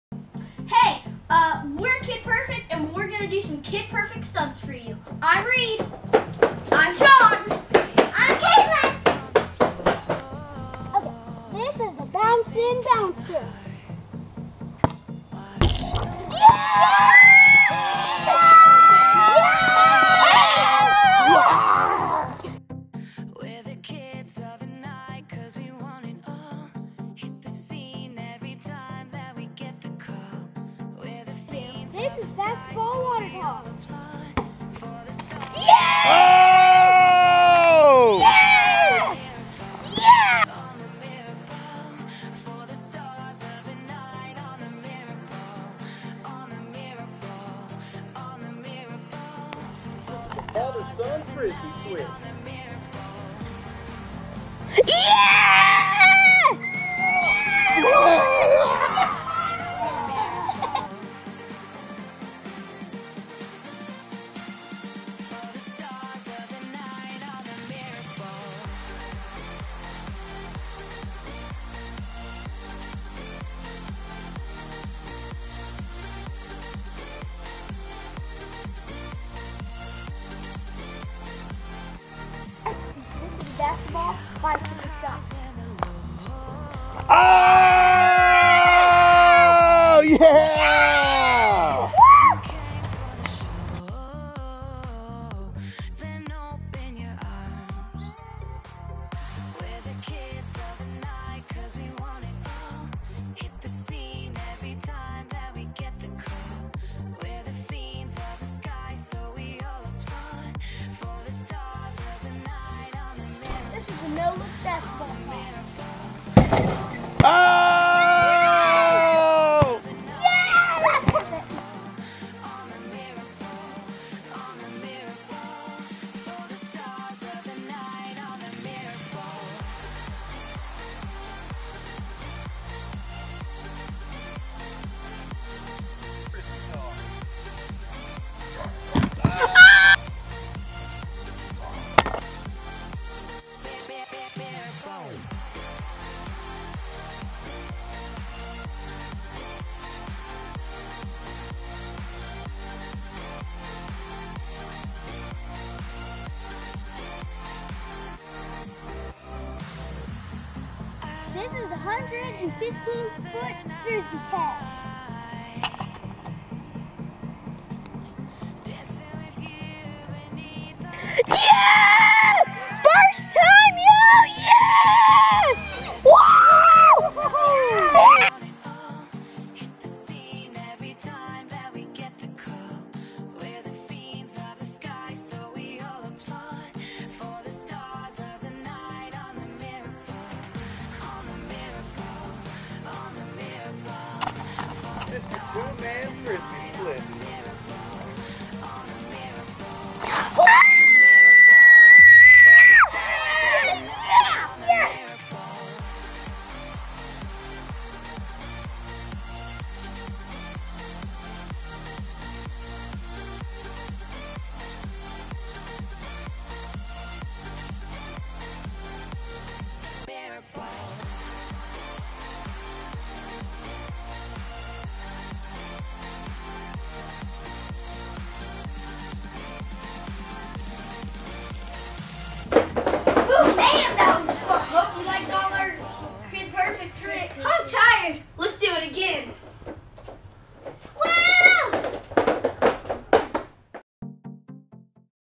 Sean Reid Kaitlyn Kid Tricks
SD Reid and Kaitlyn doing kid perfect style tricks!